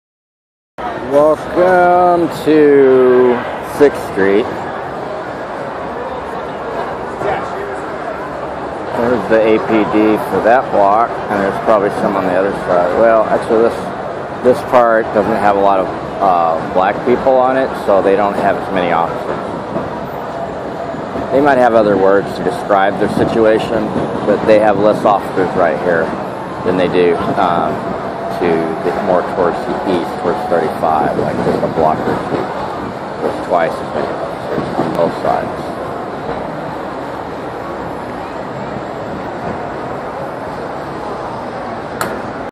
Less cops are stationed around the white clubs, more at the predomintely black clubs
Welcome to Sixth Street, Austin, Texas, U.S.A. where young men and women meet to party and meet others. It's an entertainment district spanning several miles of clubs.  You can expect good times right?
Well unfortunately only occasionally. On some occasions the Austin Police will ruin your night big-time.

So apparently, from what we are seeing, on video, the Austin Police are on the warpath, watch your backs, and watch each others. Film the Police, it's your duty and it's totally legal.

In this video the Police are stationed in smaller numbers nearer the white clubs. This is what I noticed early on. Later on I filmed the cops attacking the black clubs repeatedly on horseback.
APD is full of racist cops and cowards.
Facts clear on my videos.